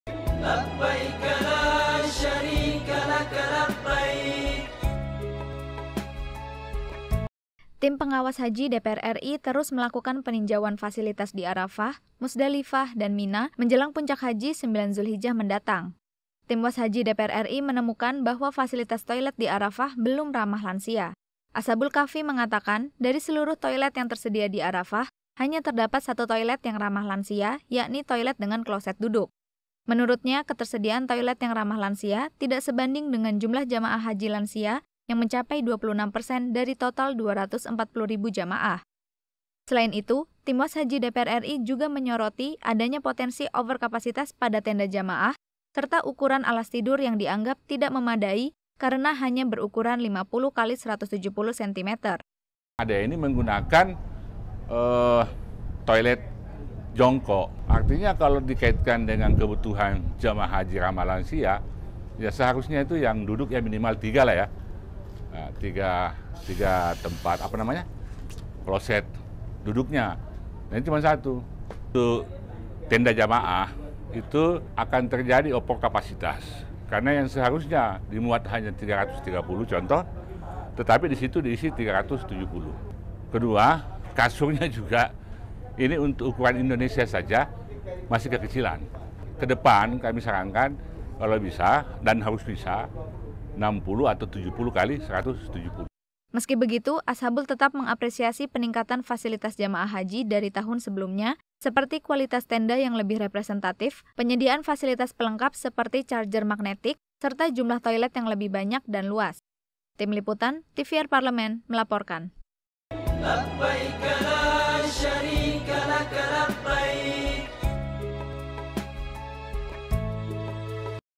Liputan Haji 2024 - Timwas Haji DPR RI : Fasilitas Toilet Di Arafah Belum Ramah Lansia
Tim Pengawas Haji DPR RI terus melakukan peninjauan Fasilitas di Arafah, Muzdalifah, dan Mina, menjelang Puncak Haji 9 Dzulhijjah mendatang. Timwas Haji DPR RI menemukan bahwa Fasilitas Toilet di arah belum Ramah Lansia.

Narasumber :

Ashabul Kahfi / Ketua Komisi VIII DPR RI / F-PAN / Sulsel I